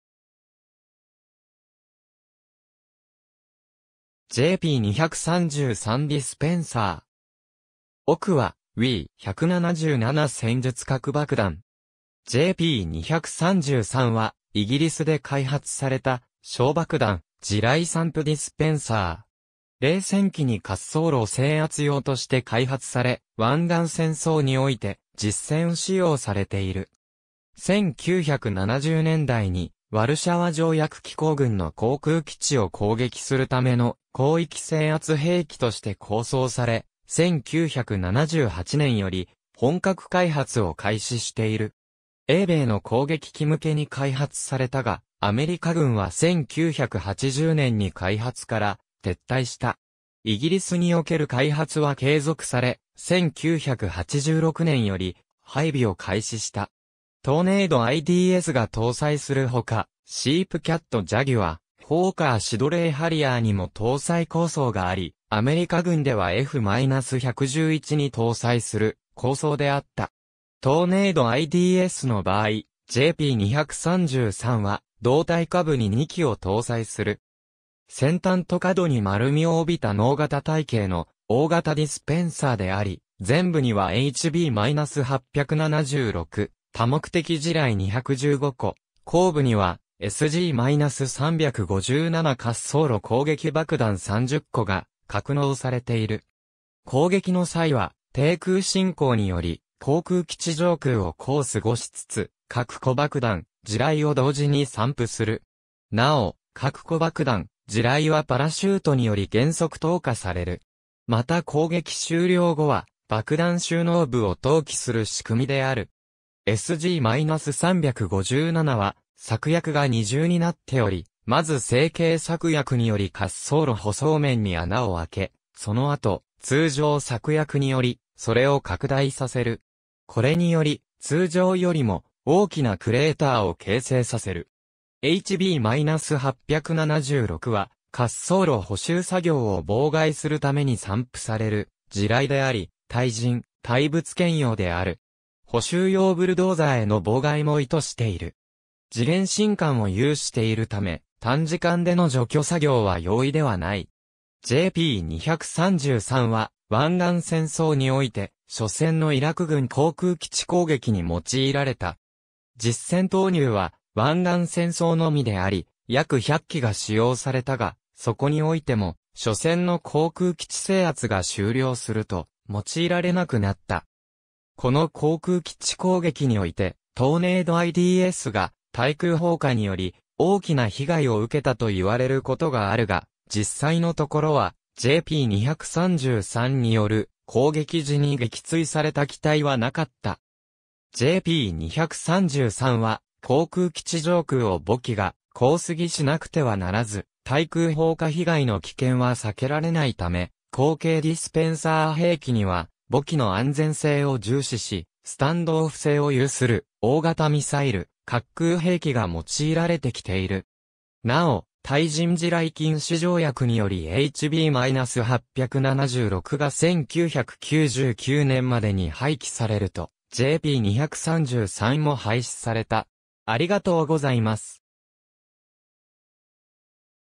JP233
JP233ディスペンサー（手前）。
奥はWE.177戦術核爆弾。
JP233はイギリスで開発された小爆弾・地雷散布ディスペンサー。
冷戦期に滑走路制圧用として開発され、湾岸戦争において実戦使用されている。
1970年代にワルシャワ条約機構軍の航空基地を攻撃するための広域制圧兵器として構想され、1978年より本格開発を開始している。
英米の攻撃機向けに開発されたが、アメリカ軍は1980年に開発から撤退した。
イギリスにおける開発は継続され、1986年より配備を開始した。
トーネード IDSが搭載するほか、SEPECAT ジャギュア、ホーカー・シドレー ハリアーにも搭載構想があり、アメリカ軍ではF-111に搭載する構想であった。
トーネード IDSの場合、JP233は胴体下部に2基を搭載する。
先端と角に丸みを帯びた直方体系の大型ディスペンサーであり、前部にはHB-876多目的地雷(2.4kg)215個、後部にはSG-357滑走路攻撃爆弾(26kg)30個が格納されている。
攻撃の際は、低空侵攻により航空基地上空を航過しつつ、各子爆弾・地雷を同時に散布する。
なお、各子爆弾・地雷はパラシュートにより減速投下される。
また攻撃終了後は、爆弾収納部を投棄する仕組みである。
SG-357は、炸薬が二重になっており、まず成形炸薬により滑走路舗装面に穴を開け、その後通常炸薬により、それを拡大させる。
これにより、通常よりも大きなクレーターを形成させる。
HB-876は滑走路補修作業を妨害するために散布される地雷であり、対人・対物兼用である。
補修用ブルドーザーへの妨害も意図している。
時限信管を有しているため、短時間での除去作業は容易ではない。
JP233は、湾岸戦争において、緒戦のイラク軍航空基地攻撃に用いられた。
実戦投入は湾岸戦争のみであり、約100基が使用されたが、そこにおいても緒戦の航空基地制圧が終了すると、用いられなくなった。
この航空基地攻撃において、トーネード IDSが対空砲火により、大きな被害を受けたと言われることがあるが、実際のところは、JP233による攻撃時に撃墜された機体は無かった。
JP233は、航空基地上空を母機が航過しなくてはならず、対空砲火被害の危険は避けられないため、後継ディスペンサー兵器には、母機の安全性を重視し、スタンドオフ性を有する大型ミサイル・滑空兵器が用いられてきている。
なお、対人地雷禁止条約によりHB-876が1999年までに廃棄されると、JP233も廃止された。